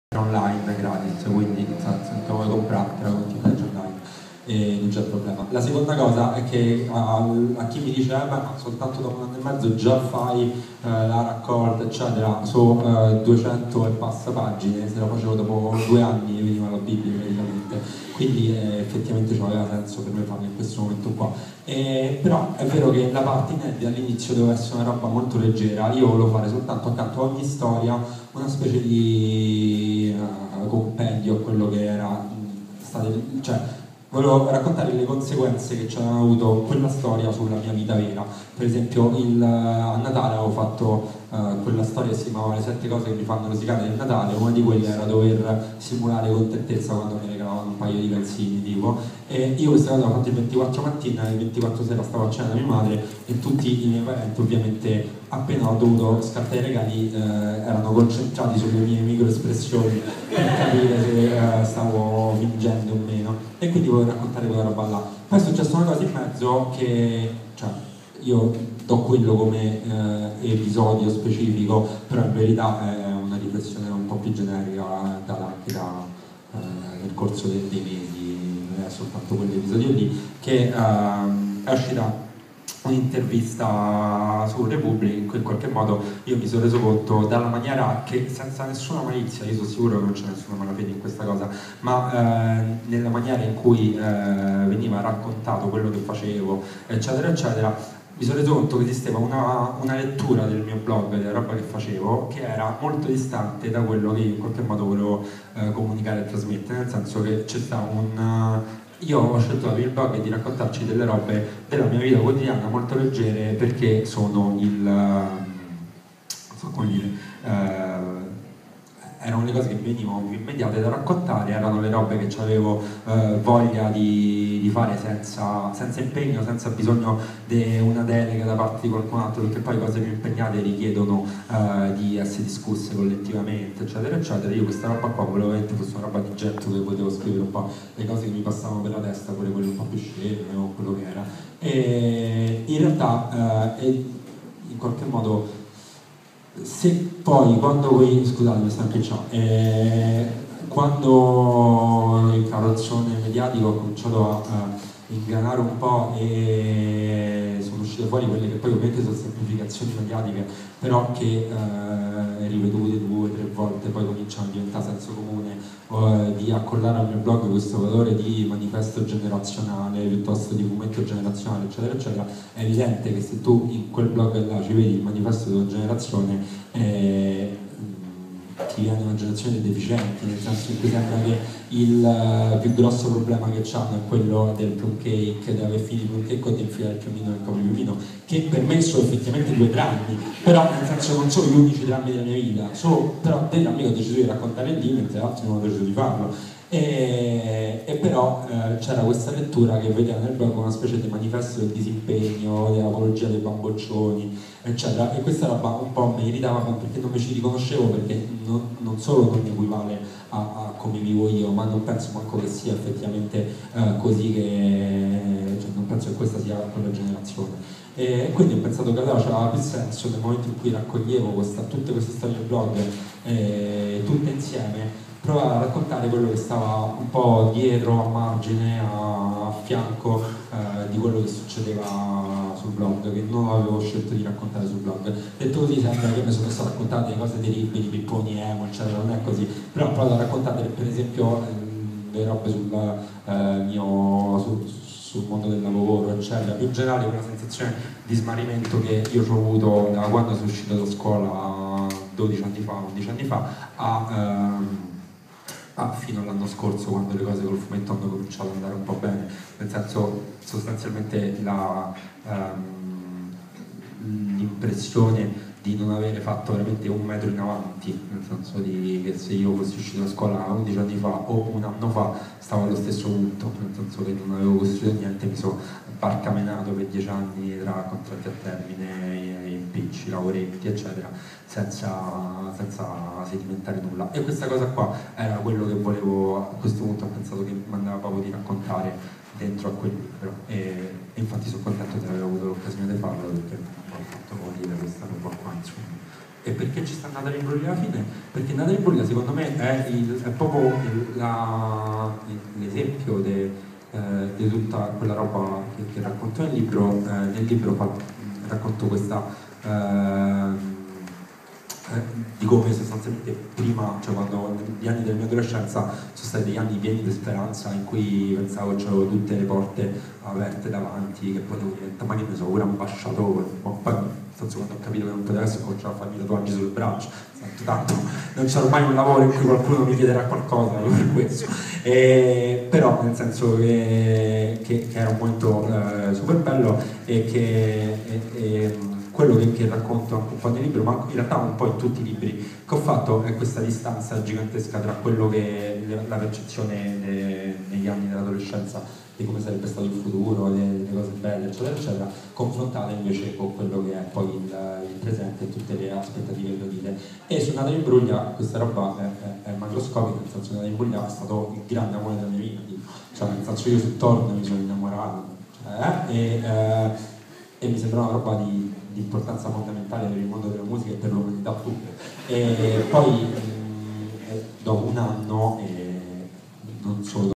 Comics TV: La coscienza di Zero, i video dell'inaugurazione della mostra di Zerocalcare (4/4)
Dal 16 giugno al 31 luglio 2013 WOW Spazio Fumetto, in collaborazione con Bao Publishing, dedica una mostra a uno dei più interessanti fenomeni del fumetto italiano contemporaneo: Zerocalcare, l'ironico, surreale e simpaticissimo personaggio che ha conquistato migliaia di lettori grazie al modo graffiante con cui racconta la realtà di tutti i giorni.